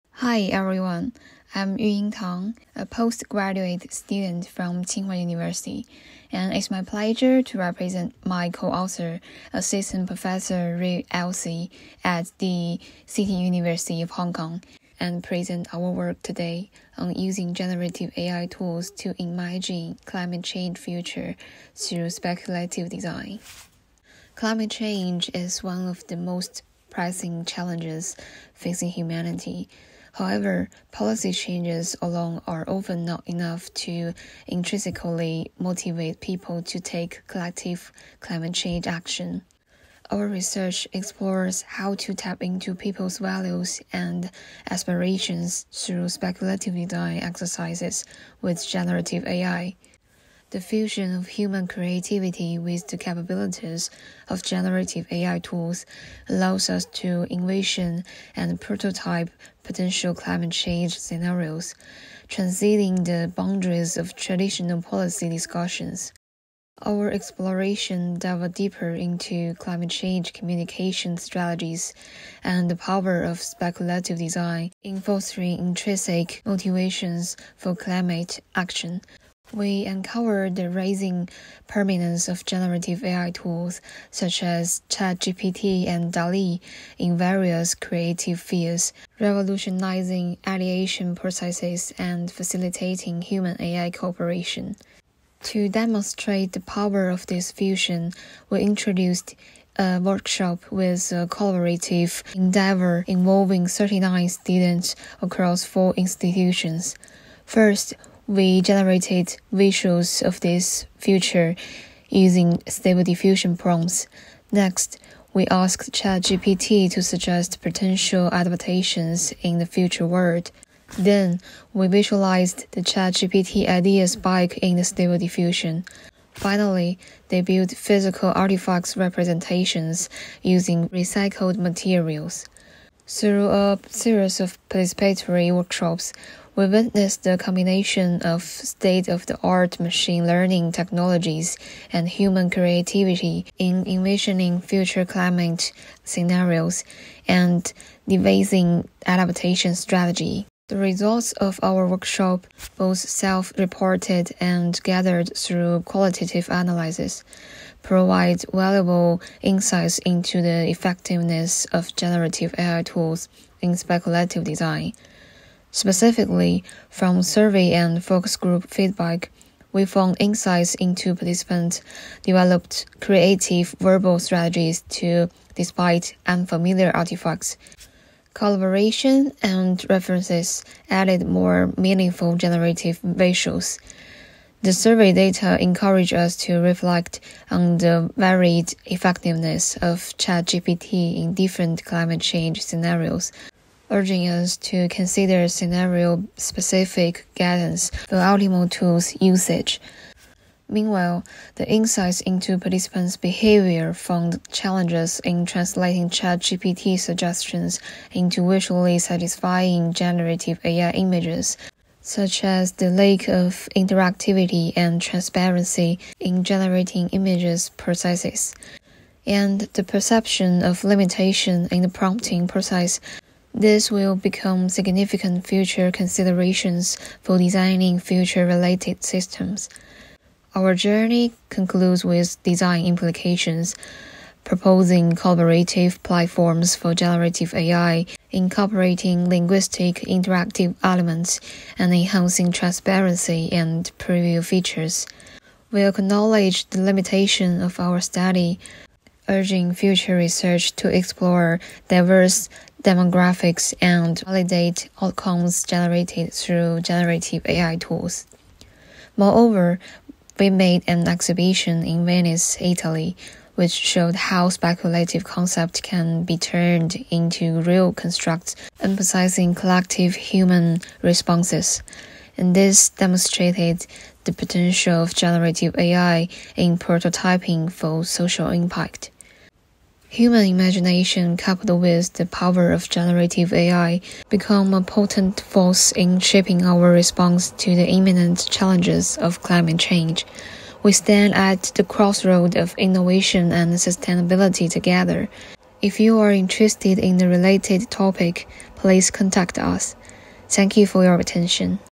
Speculative Design with Generative AI paper presentation @ ARTECH'23 @ RAY LC @ Yuying Tang
Speculative Design with Generative AI:
Applying Stable Diffusion and ChatGPT to Imagine Climate Change Futures
by RAY LC and Yuying Tang